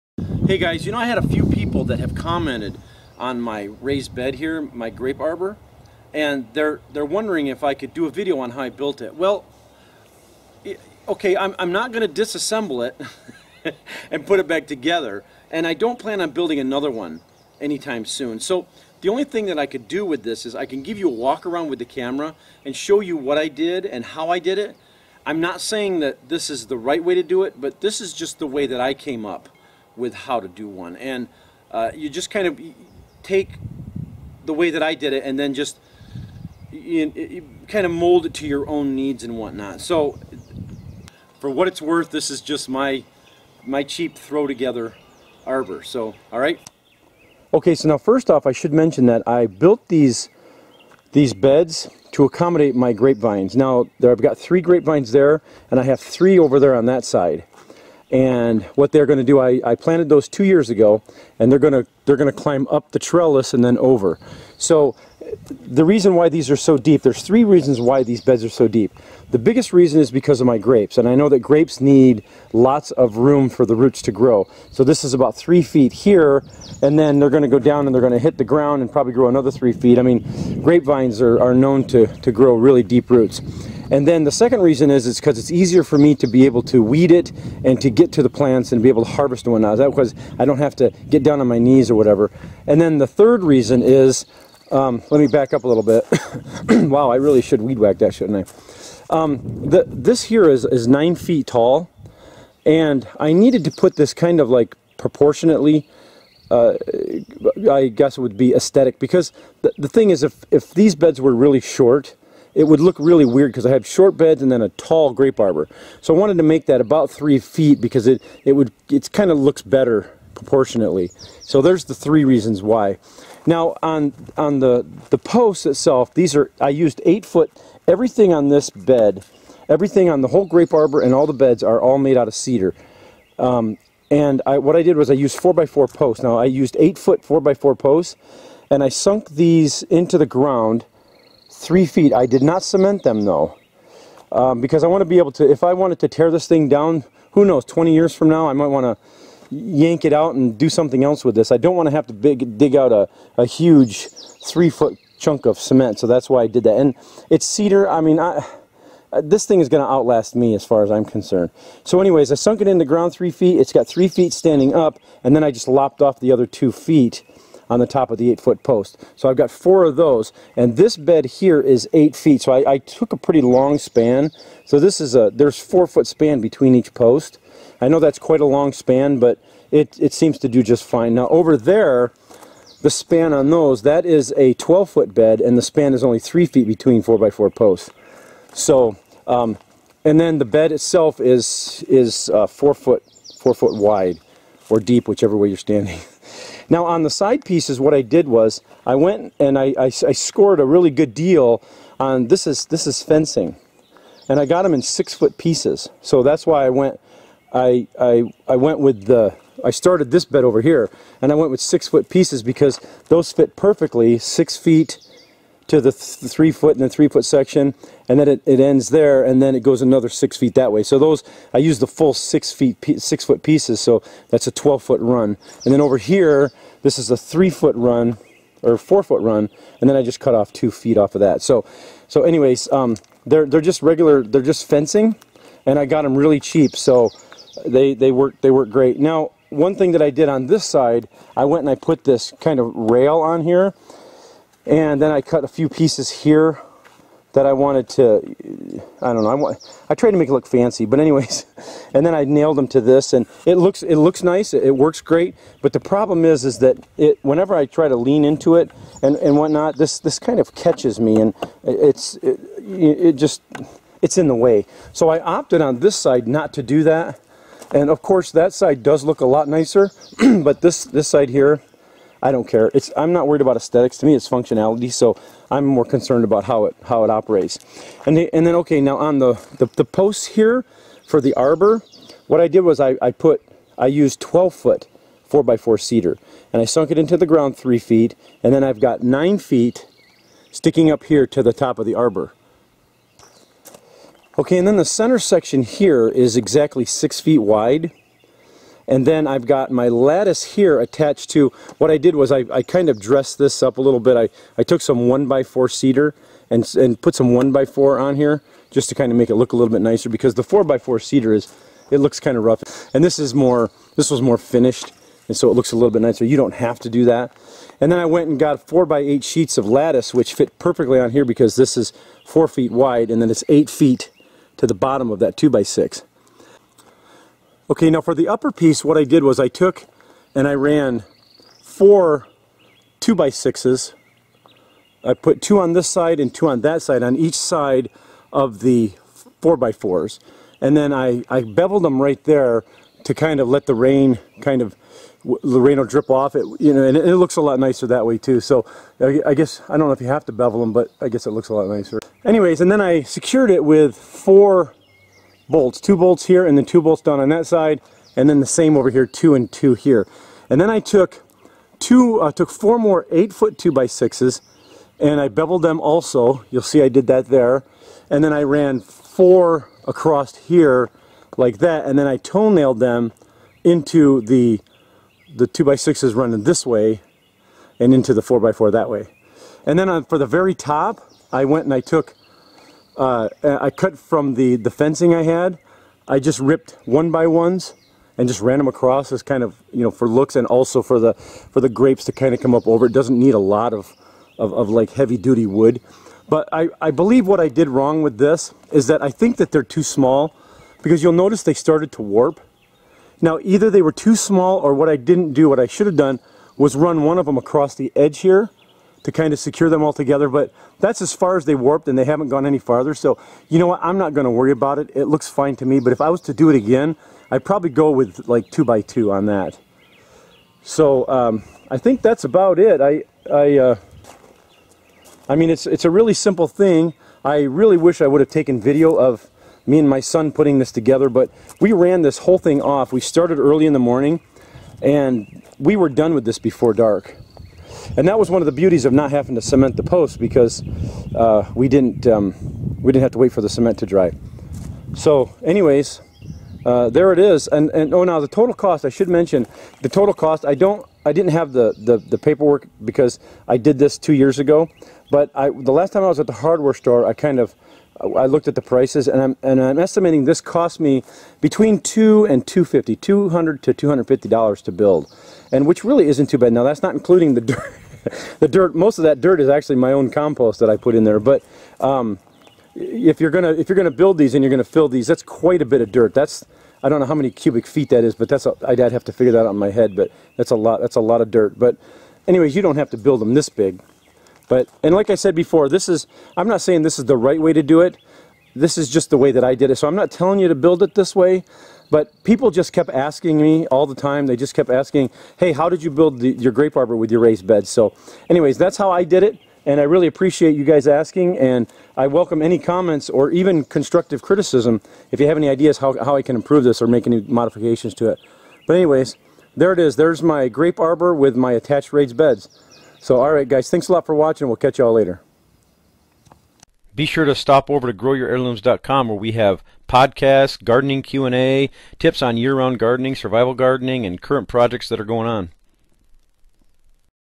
Grape Arbor With Attached Raised Beds
Do you want to build a grape arbor?
After multiple requests, I decided to do a walk around of my grape arbor with the attached raised beds.